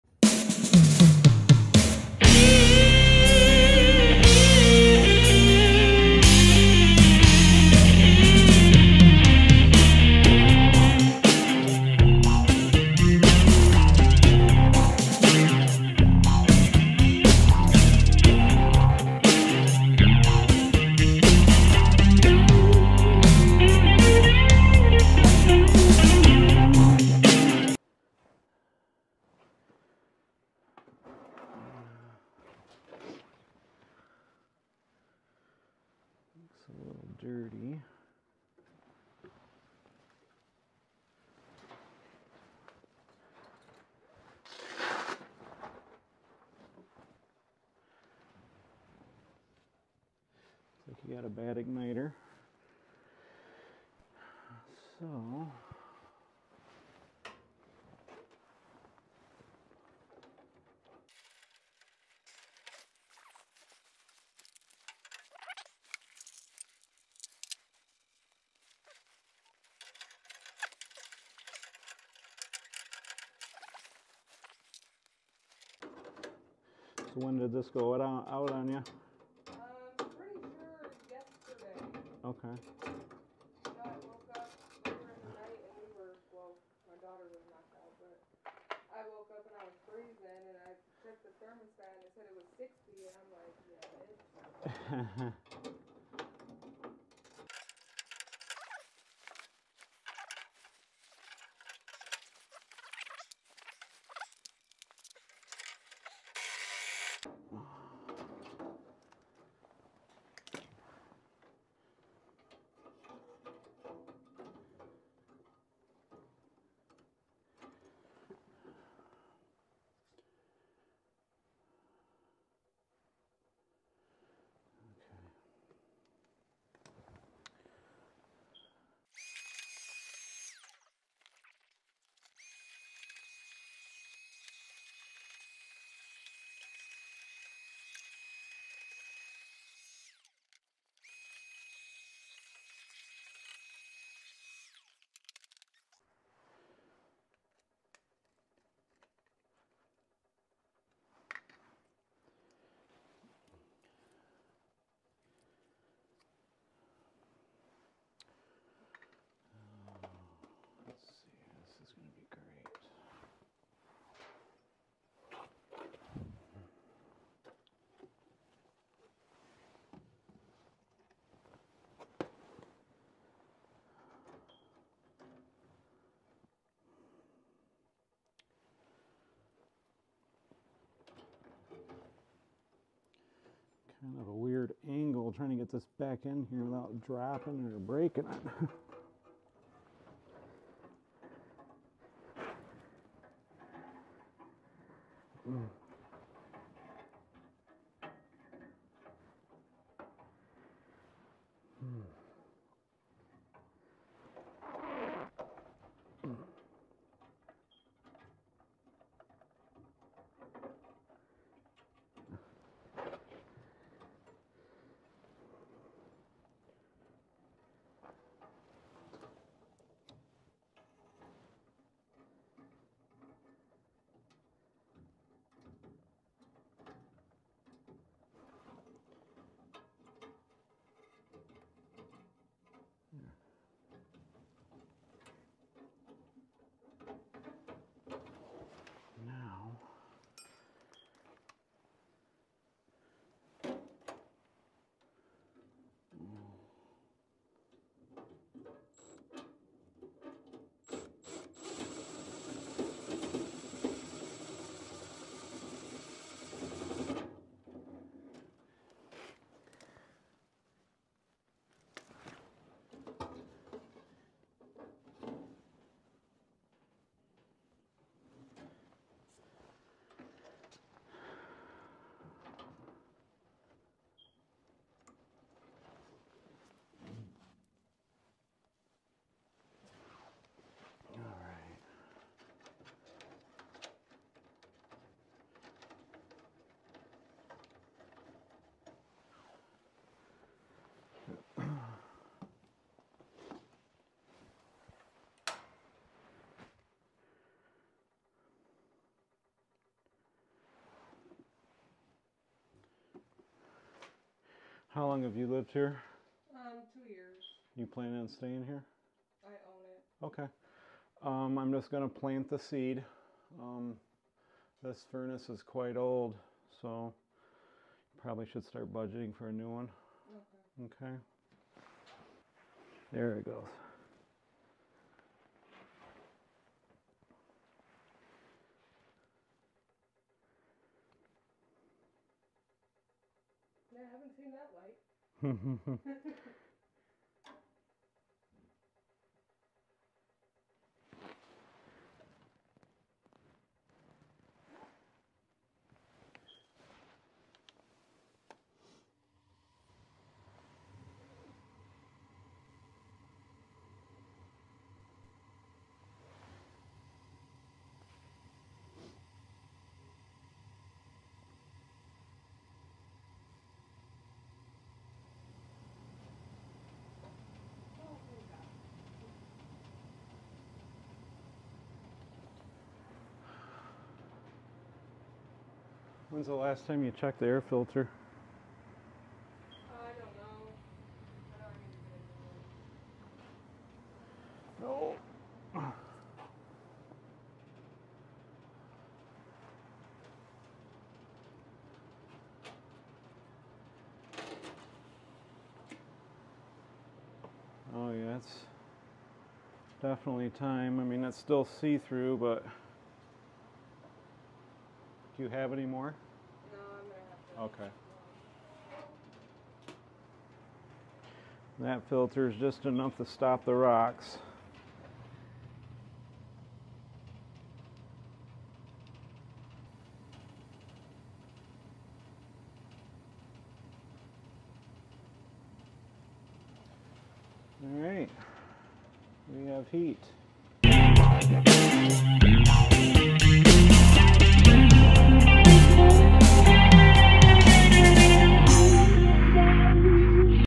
"You Won't Believe How We Fixed This Rheem Furnace!"
Are you having trouble with your rheem furnace not generating heat? In this video, we'll show you how we fixed it!
Fixing a Rheem furnace isn't always easy, but we managed to do it in a short amount of time. We'll be more than happy to help you out!

I hope you enjoyed it. Please leave any thoughts, feedback, comments,  Constructive criticism, or advice in the comment section. Need to get in touch?